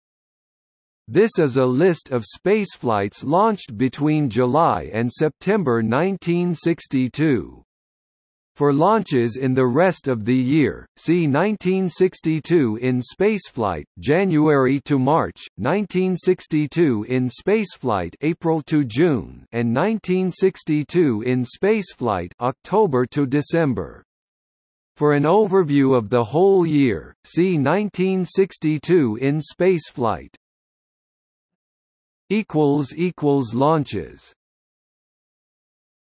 1962 in spaceflight (July–September) | Wikipedia audio article
- Socrates

SUMMARY
This is a list of spaceflights launched between July and September 1962. For launches in the rest of the year, see 1962 in spaceflight (January–March), 1962 in spaceflight (April–June) and 1962 in spaceflight (October–December). For an overview of the whole year, see 1962 in spaceflight.